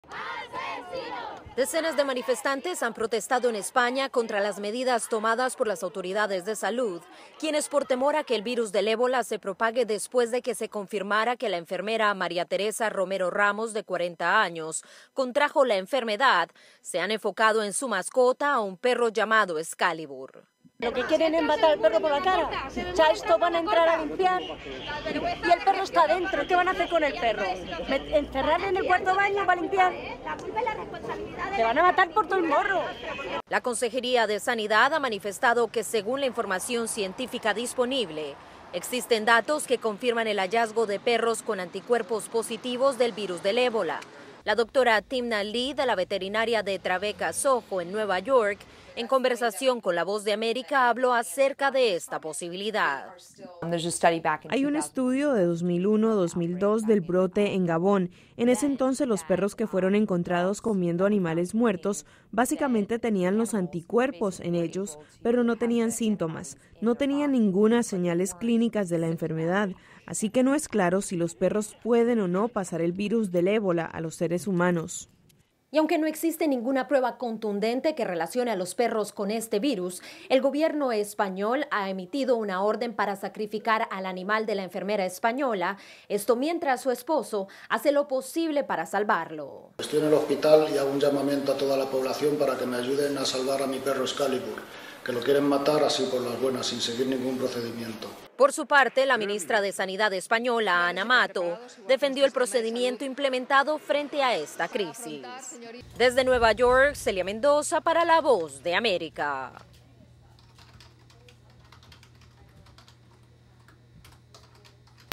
Ébola en Perros
La decisión de las autoridades sanitarias de sacrificar al perro de la paciente de ébola en España ha despertado fuertes emociones por parte de los defensores de animales, así como dudas acerca de esta posibilidad.